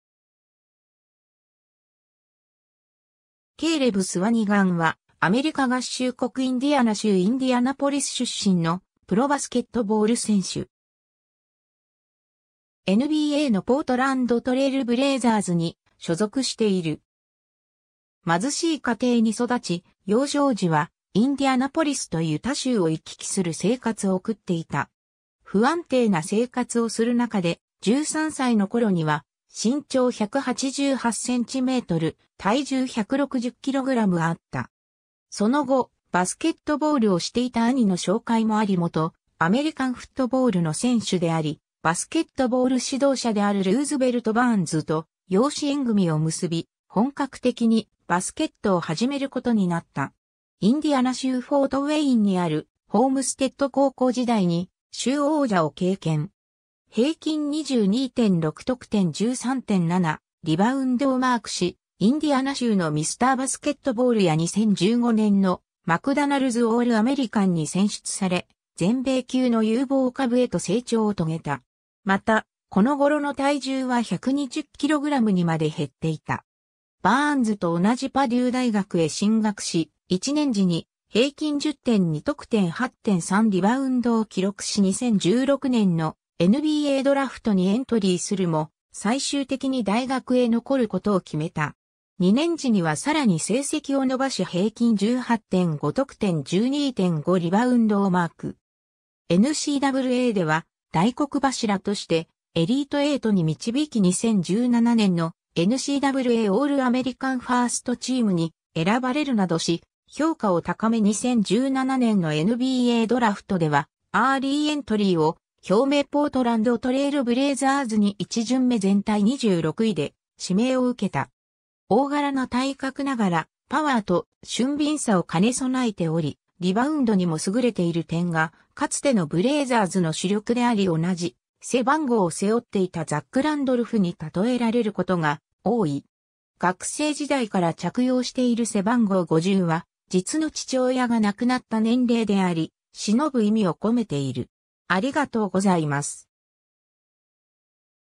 ケイレブ・スワニガン
ケイレブ・スワニガン (Caleb Swanigan 1997年4月18日 - )は、アメリカ合衆国インディアナ州インディアナポリス出身のプロバスケットボール選手。
NBAのポートランド・トレイルブレイザーズに所属している。
 貧しい家庭に育ち、幼少時はインディアナポリスとユタ州を行き来する生活を送っていた。
不安定な生活をする中で、13歳の頃には身長188cm・体重160kgあった。
その後、バスケットボールをしていた兄の紹介もあり元アメリカンフットボールの選手であり、バスケットボール指導者であるルーズベルト・バーンズと養子縁組を結び、本格的にバスケットを始めることになった。
 インディアナ州フォートウェインにあるホームステッド高校時代に州王者を経験。
平均22.6得点 13.7リバウンドをマークし、インディアナ州のMr.バスケットボールや2015年のマクダナルズ・オール・アメリカンに選出され、全米級の有望株へと成長を遂げた。
また、この頃の体重は120kgにまで減っていた。
 バーンズと同じパデュー大学へ進学し、1年次に平均10.2得点 8.3リバウンドを記録し2016年のNBAドラフトにエントリーするも最終的に大学へ残ることを決めた。
2年次にはさらに成績を伸ばし平均18.5得点 12.5リバウンド(全米第2位)をマーク。
NCAAでは大黒柱としてエリートエイトに導き2017年のNCAAオールアメリカンファーストチームに選ばれるなどし評価を高め2017年のNBAドラフトではアーリーエントリーを表明ポートランド・トレイルブレイザーズに1巡目全体26位で指名を受けた。
 大柄な体格ながらパワーと俊敏さを兼ね備えており、リバウンドにも優れている点が、かつてのブレイザーズの主力であり同じ背番号を背負っていたザック・ランドルフに例えられることが多い。
 学生時代から着用している背番号50は実の父親が亡くなった年齢であり、偲ぶ意味を込めている。